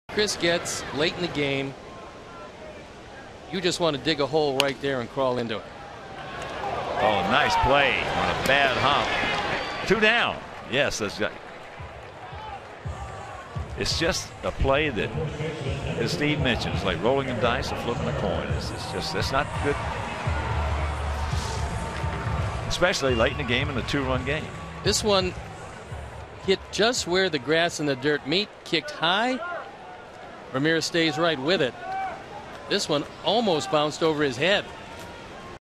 CWS@KC: Alexei reaches high to haul in a tough hop
8/20/13: Alexei Ramirez sticks with it to make a great play on a tough hop on a ball off the bat of Jamey Carroll in the eighth inning